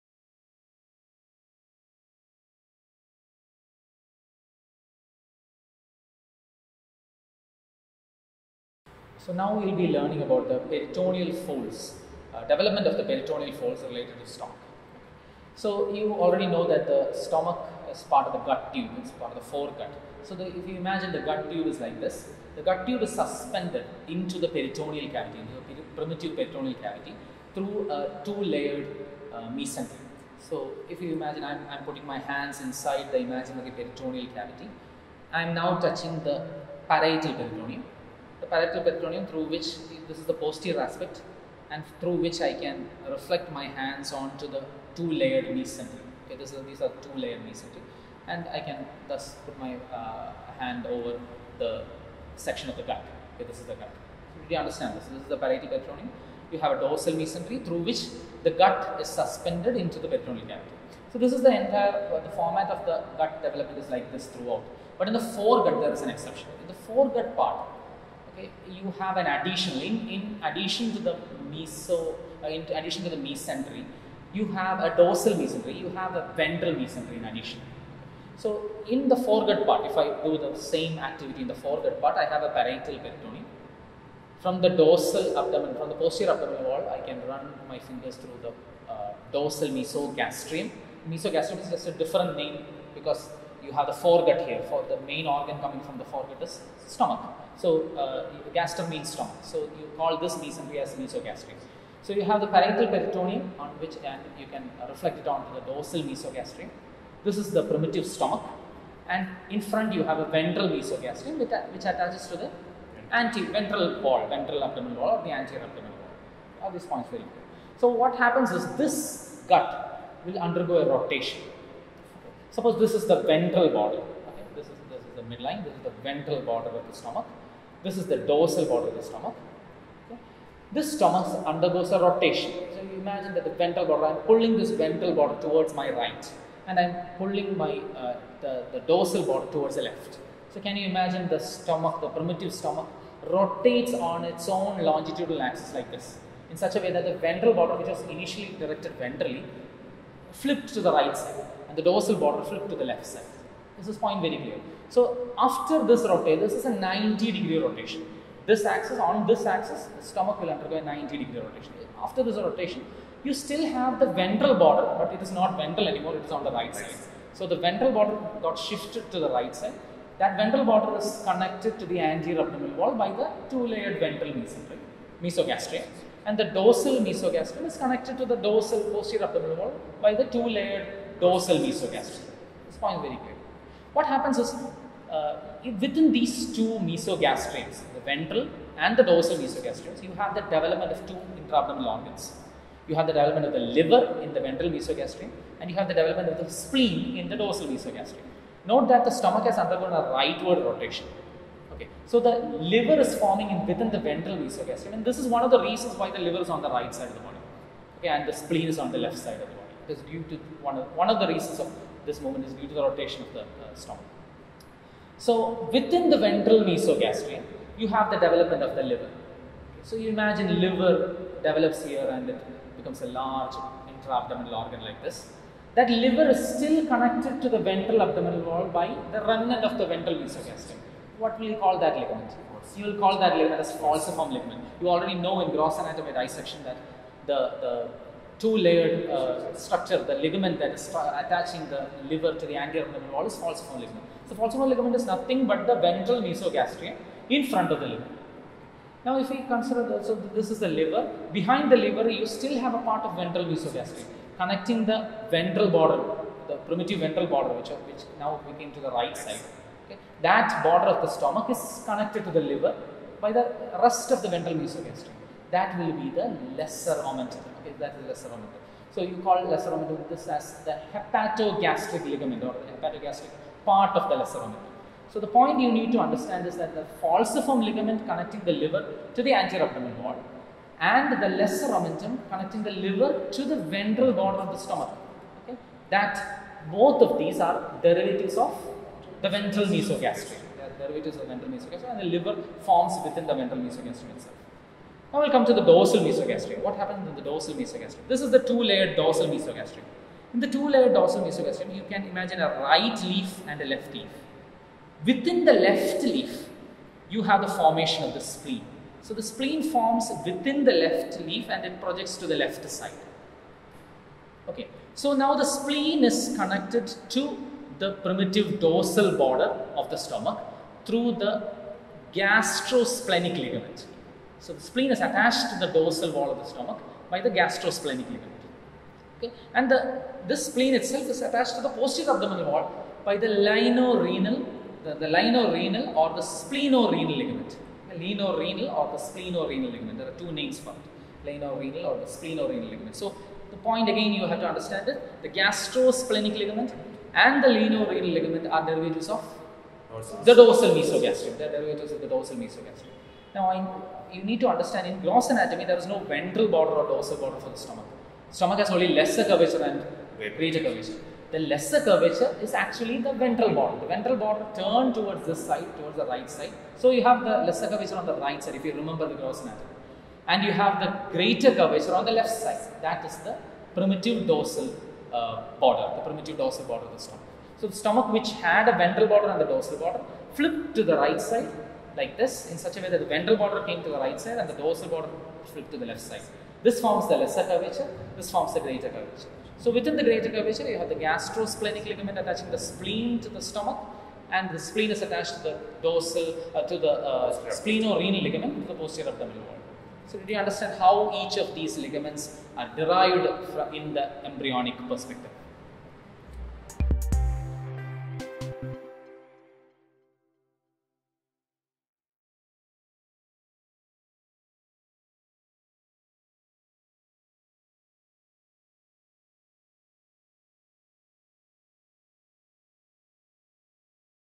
Stomach embryologic rotation | Air Anatomy
Video in which 'Air Anatomy' gestures alone are used to make the students imagine and understand the embryologic phenomenon of stomach rotation and the formation of 'meso-gastriums' and their mature derivatives.

Air Anatomy is a highly involving, flexible and literally 'no cost' method to learn spatial content.

Follow this channel for more 'Air Anatomy' videos!